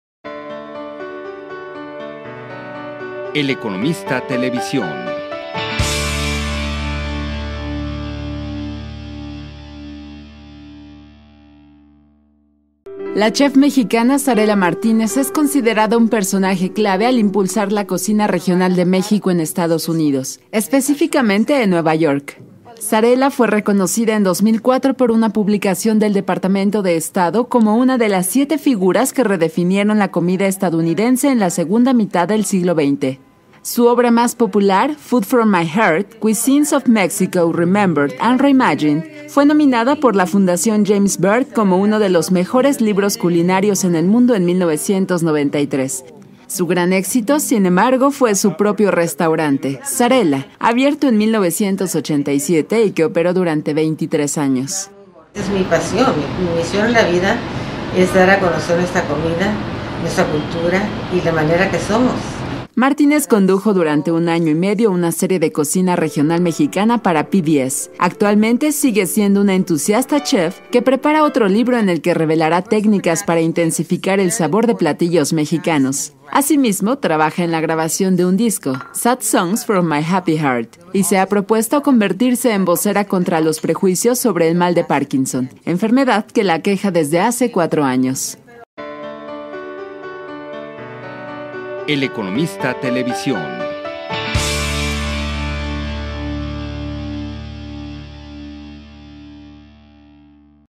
Consideran a Zarela Martínez pionera de cocina mexicana en EUA
La chef mexicana Zarela Martínez es considerada un personaje clave en impulsar la cocina regional de México en Estados Unidos, específicamente en Nueva York, donde por más de dos décadas mantuvo un popular restaurante, publicó libros y realizó una serie de televisión.